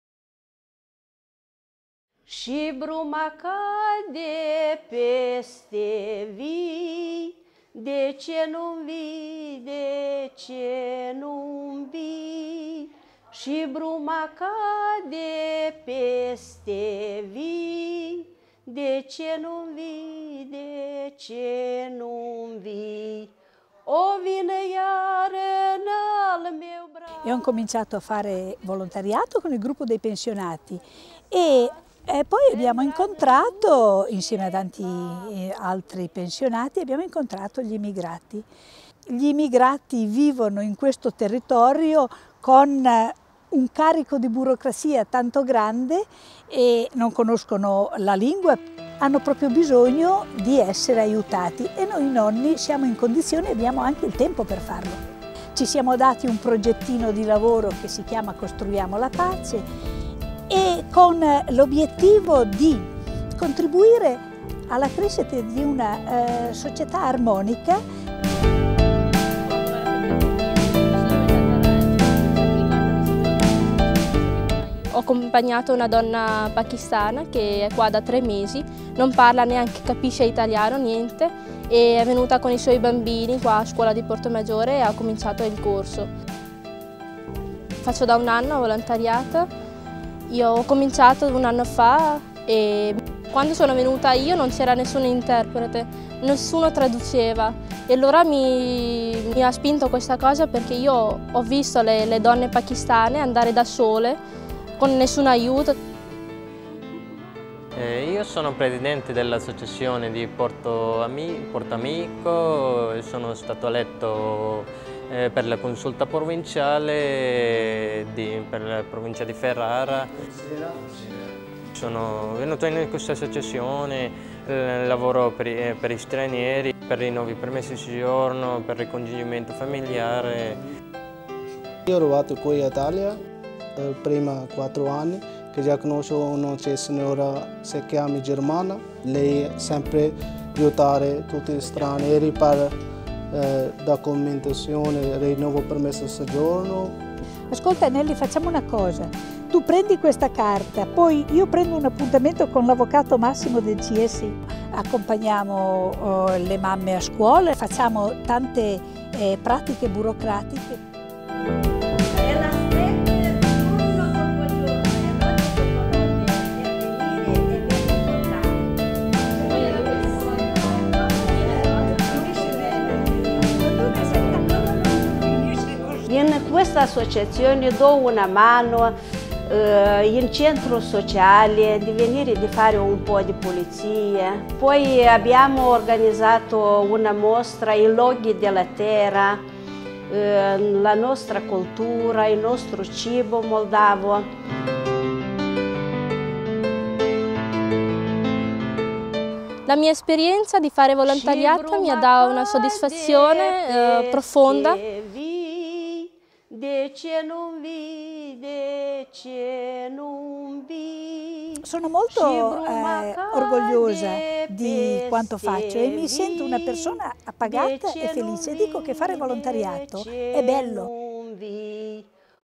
Stranieri
Associazione di Volontoriato che ha come scopo quello di aiutare gli immigrati ad inserirsi nella nostra società innquesto caso nella comunità di Portomaggiore